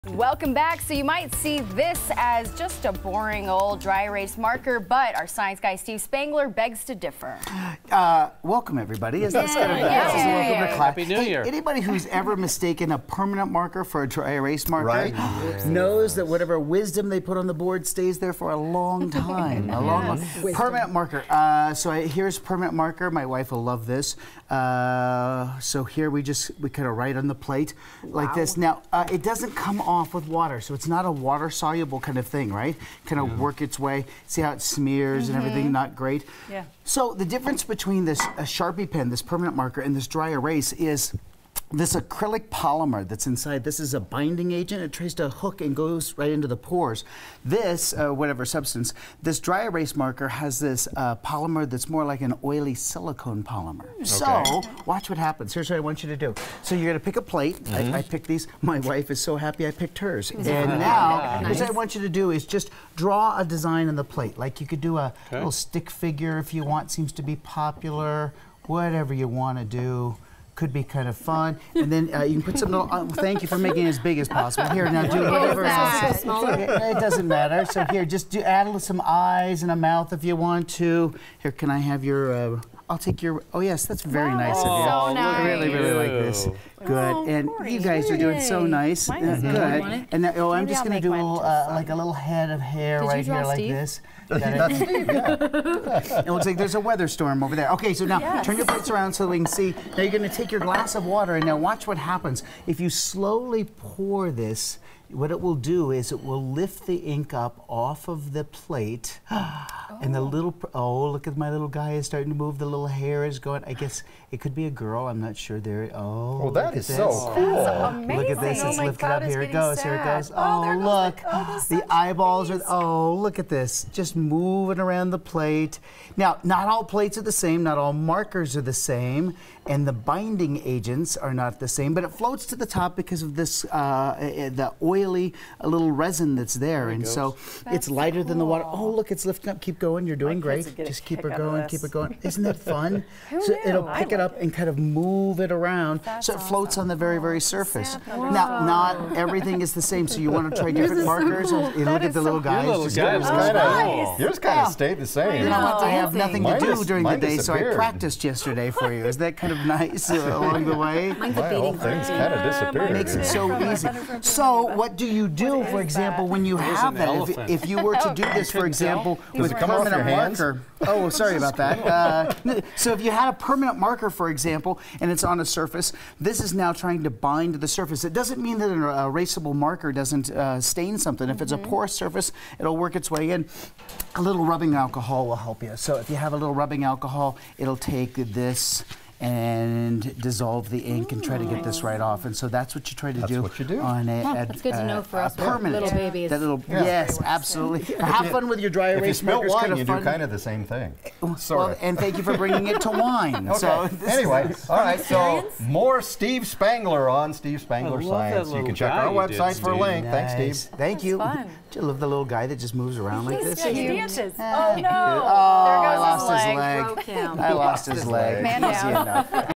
Spangler Science: This fun trick uses dry erase markers
You'll never look at a dry erase marker the same way after spending a few minutes with our science guy Steve Spangler.

9NEWS (KUSA) is located in Denver, Colorado.